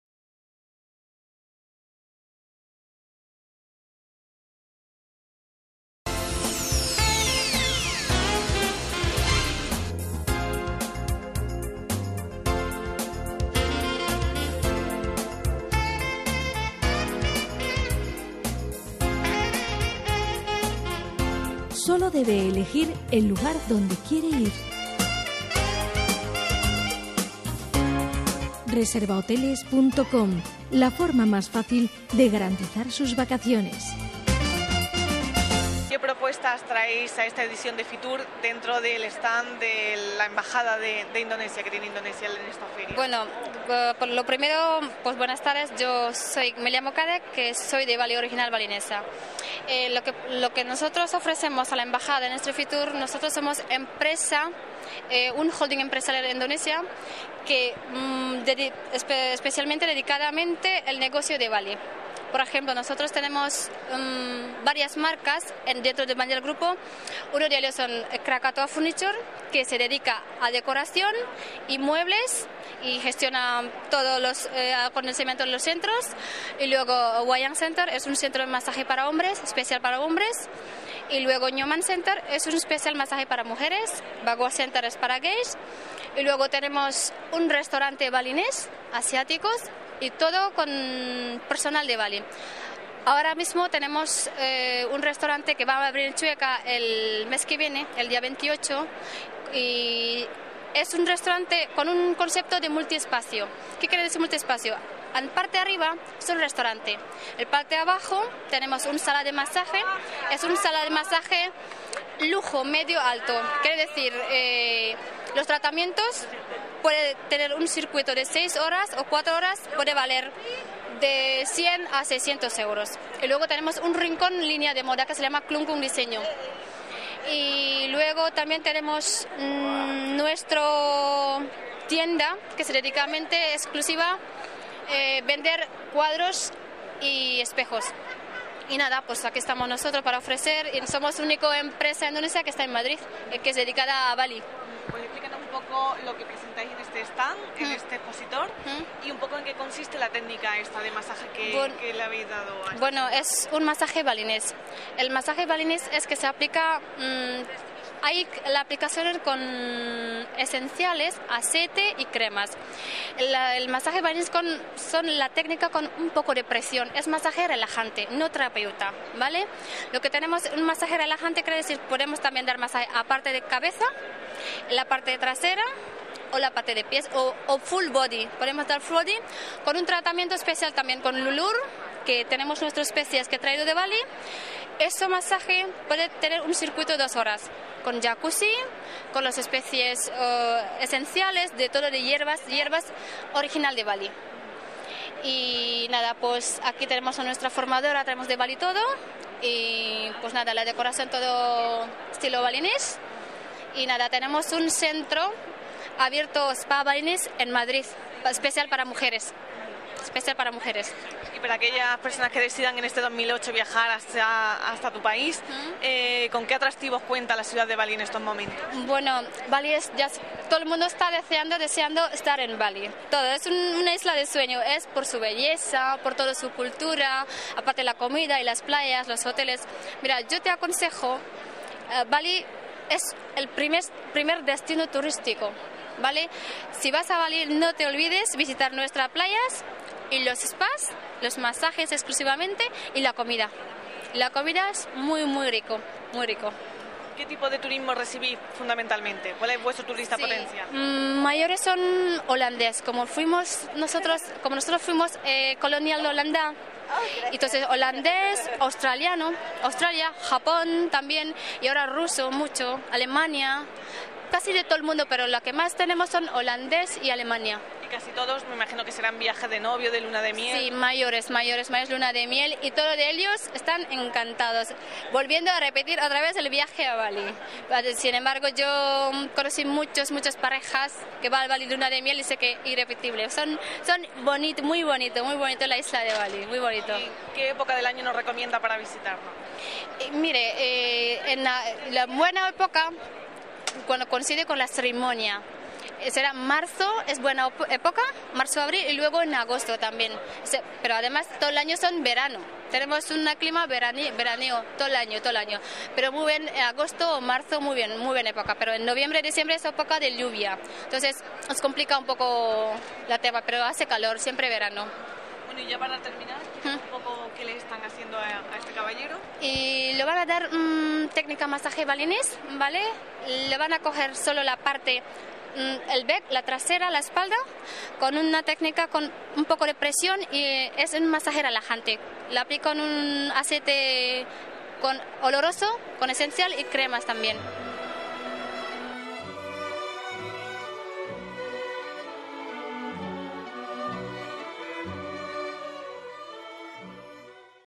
Indonesia, un destino exótico en Fitur 2008
Kadek Endang, The Banjar Group Indonesian Holding, mostró en el stand de Indonesia en Fitur 2008 su oferta de restauración, decoración y masajes a todos los que se acercaron a su expositor. Asimismo, habló de los atractivos que le ofrece al visitante la ciudad de Bali.